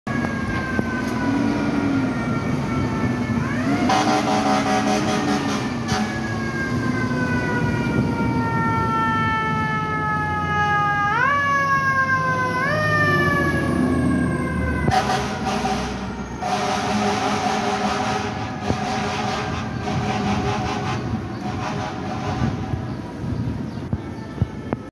FDNY Engine 273 Spare responding to a class 3 alarm
Engine 273 response to Box9411 at 133-20 Avery Ave for a class 3 alarm. Engine 273 gave the final signal a 10-18 for a 10-26

This is video was taken on 6/14/24